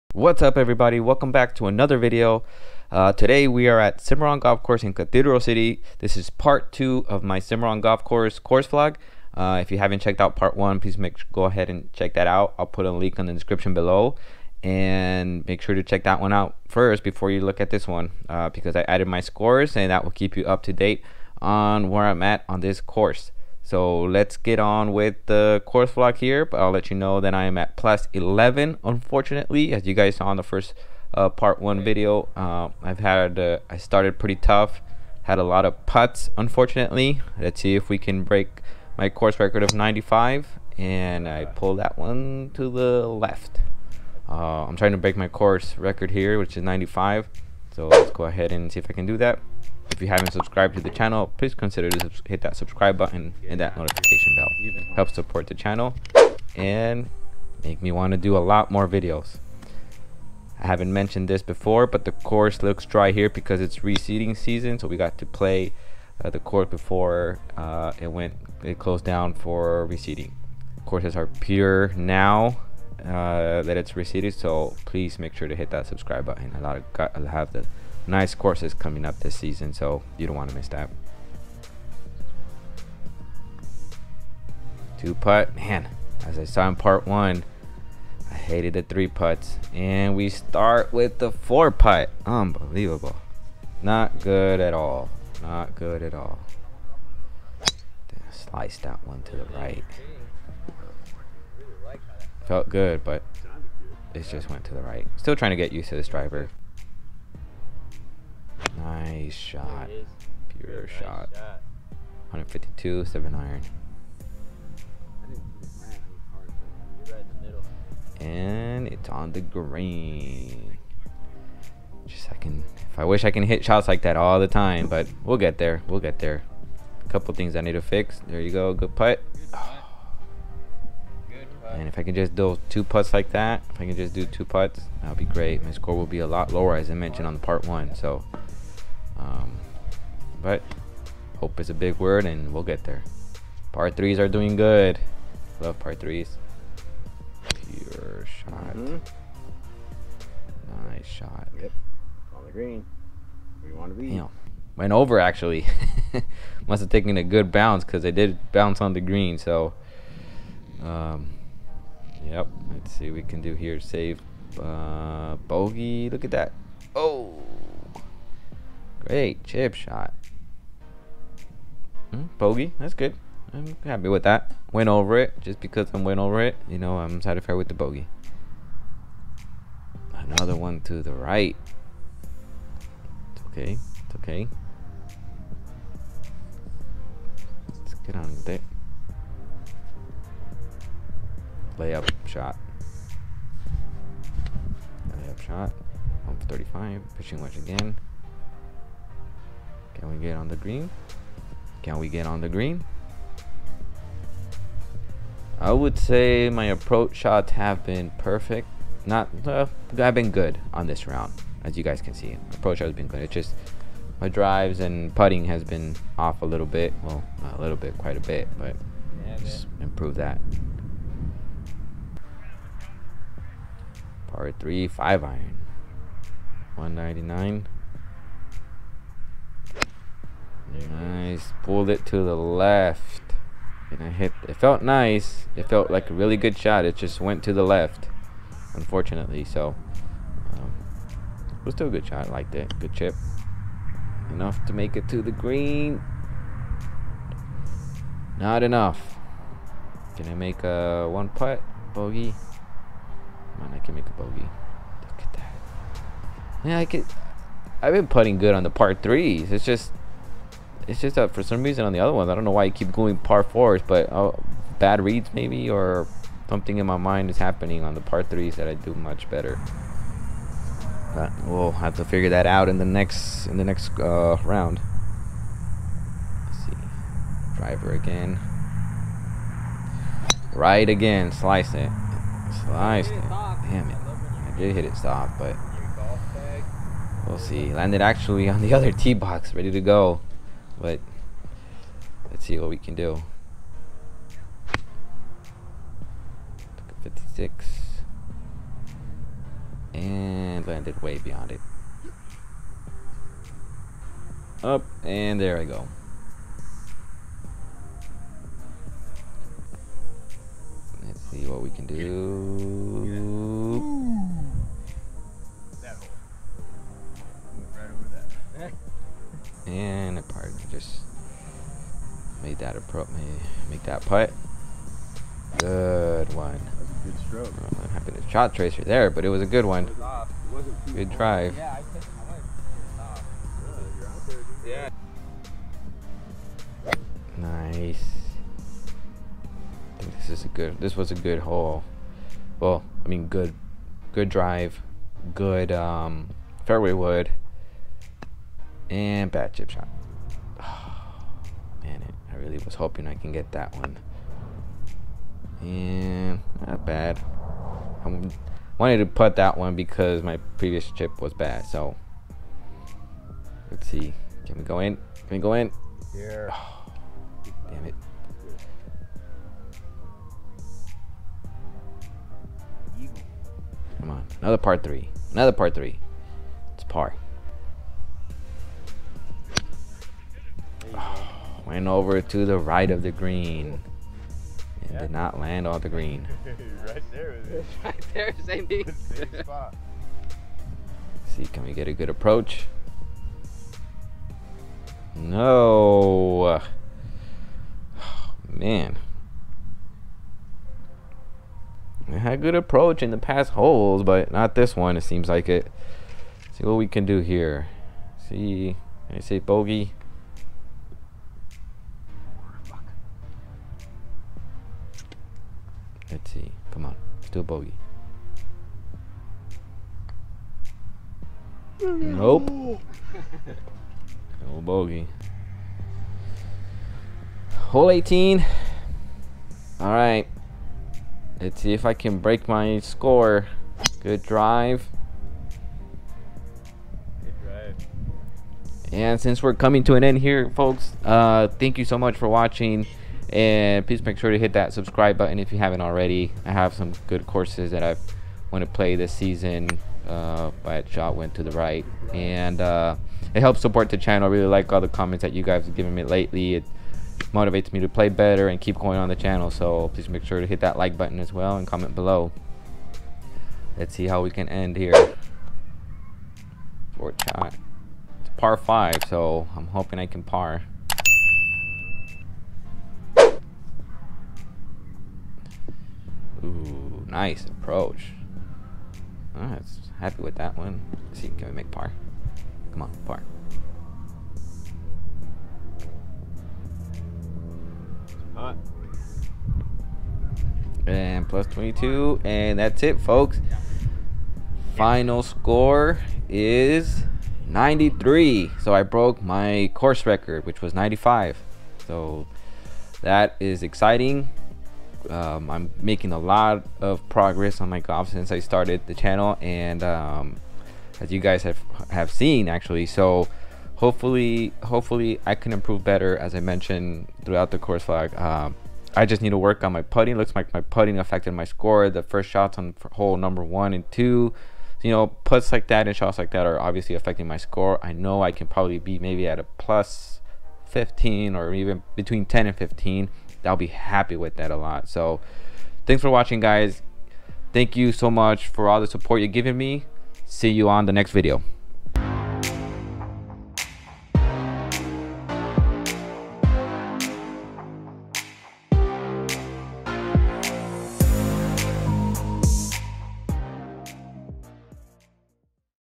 Can You Break Your Own Course Record? | Cimarron Golf Course | PART 2
Can I Break My Own Course Record... Stay tune and find out.. And don't forget to Hit that Subscribe Button, Like and Comment Below what is the current course record you are trying to break?

Equipment: GoPro 8 (Settings: 1080 - Linear - 60 FPS)
Video Editing Software: Adobe Premiere Pro
Other Software Used: Shot Tracer

Thank you, Fernando